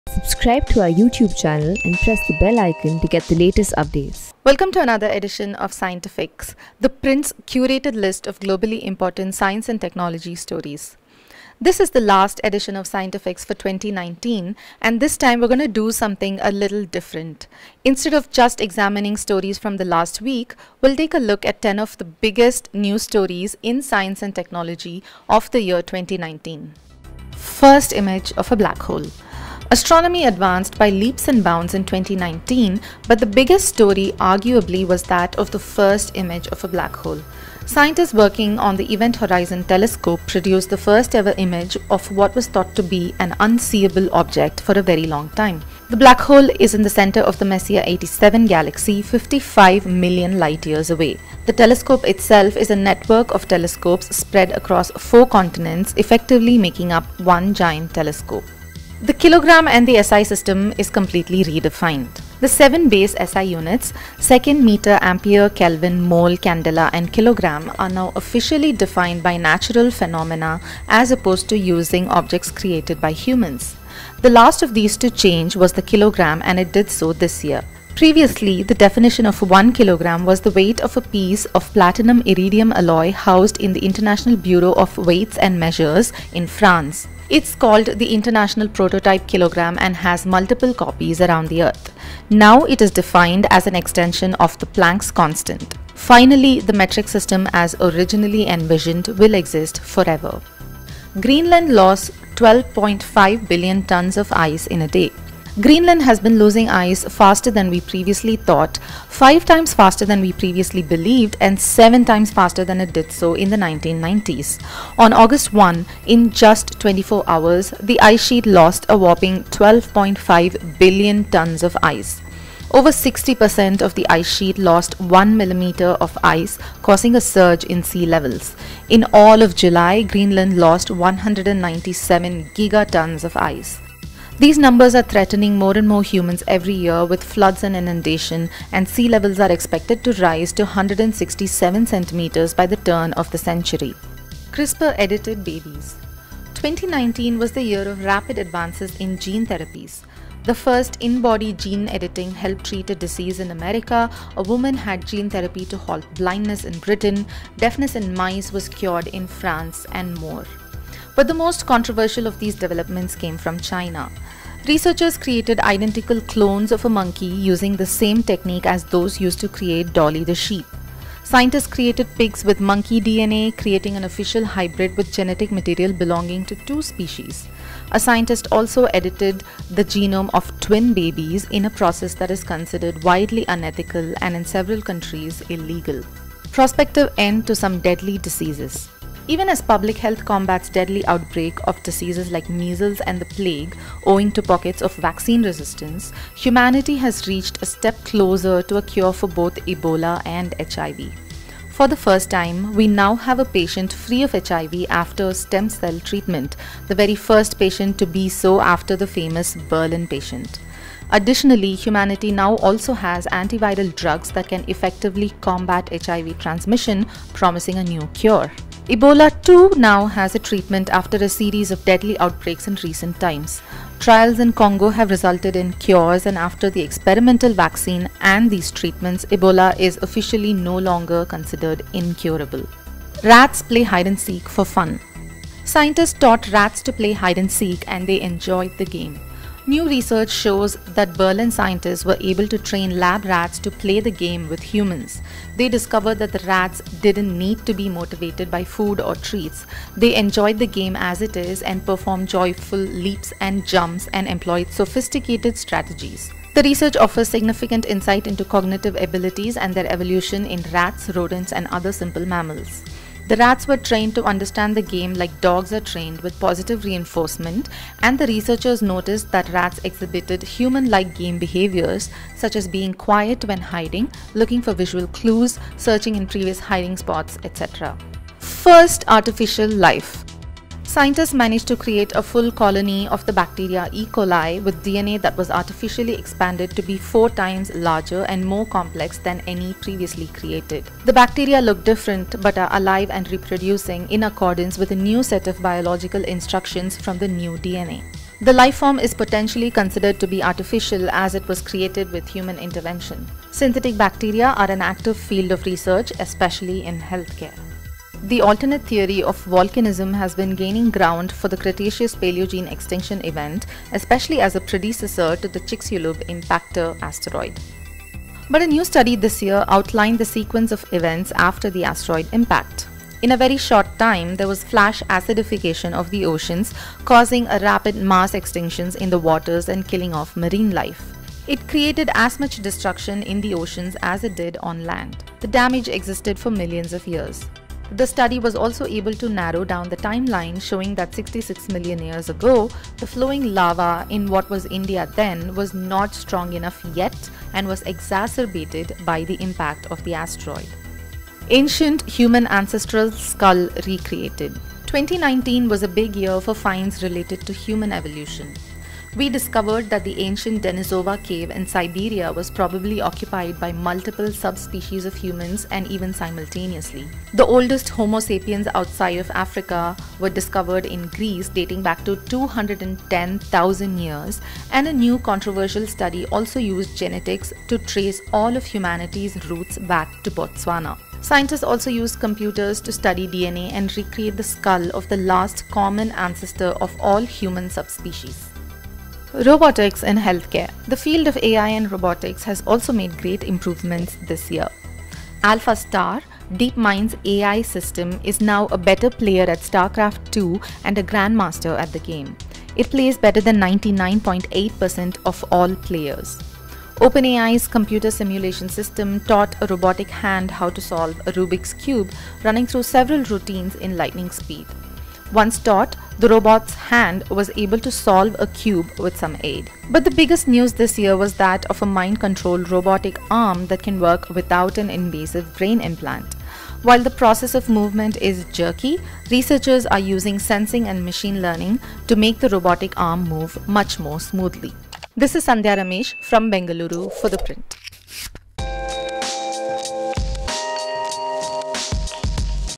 First image of a black hole, gene-edited babies, record-breaking ice melt
ScientiFix is ThePrint’s weekly curated list of globally important science stories. ThePrint's Sandhya Ramesh brings you the most interesting science stories of 2019 in this episode of Scientifix. See here the first photos of the Black Hole, what is the weight of a kilogram in nature and why Chinese scientists cloned monkeys using gene-editing techniques.